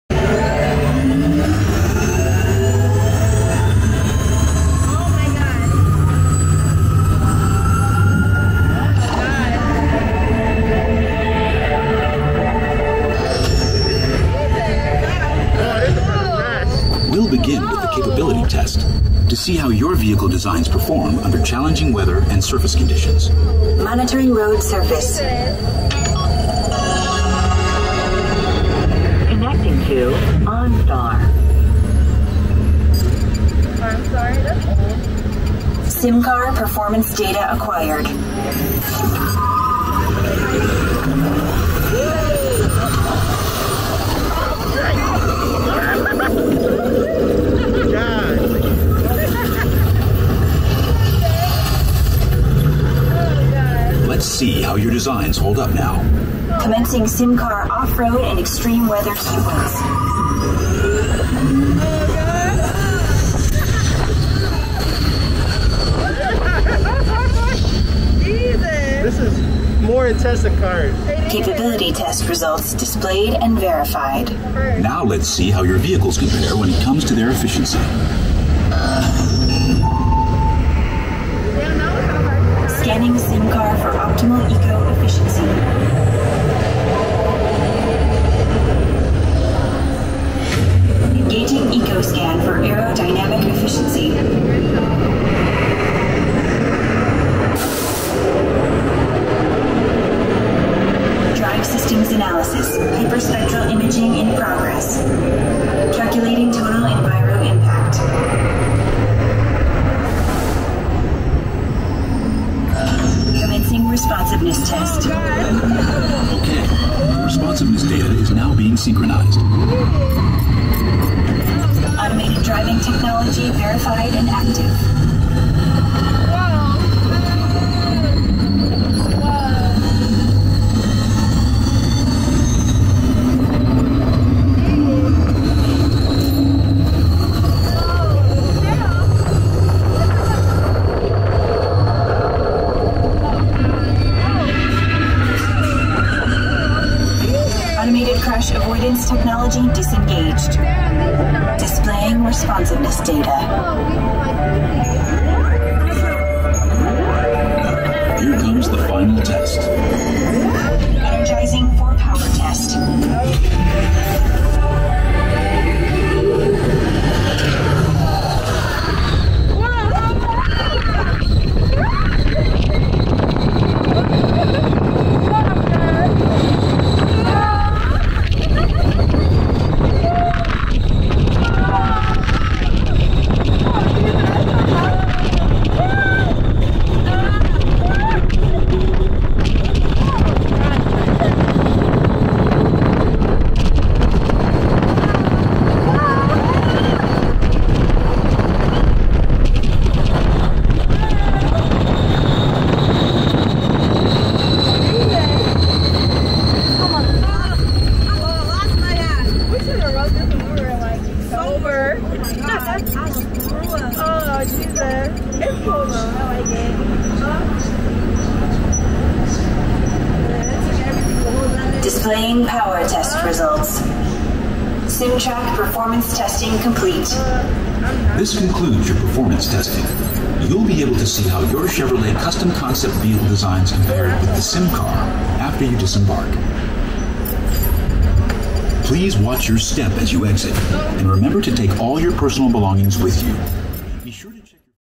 Test Track Presented by Chevrolet
We ride Test Track finally. A high-speed slot car dark ride in Epcot. The last time we were in line and it broke down. It's pretty intense for a Disney ride.